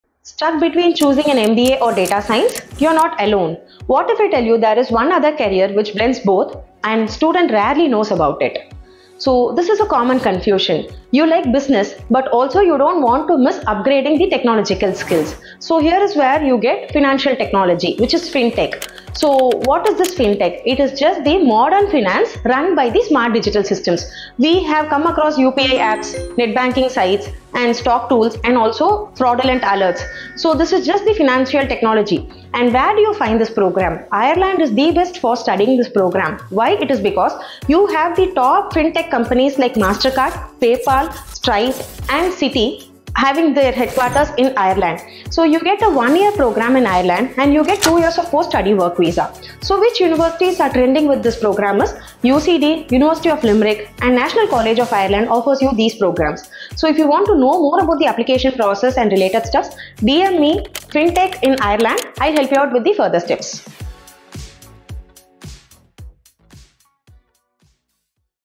Fintech in Ireland 🇮🇪 | Best Master's Programs, Jobs, PSW Explained!
Thinking about combining Finance and Tech?
In this YouTube Short, discover why Ireland is one of the hottest destinations for Fintech programs in Europe!

✔ Top Fintech Master's programs
✔ Career roles: Fintech Analyst, Blockchain Consultant, etc.
✔ Eligibility for non-CS background
✔ 2-year post-study work visa
✔ Trending programs: UCD, NCI, TUDublin

🎓 DM @foredoverseas for full guidance, brochures, and your personalized university list!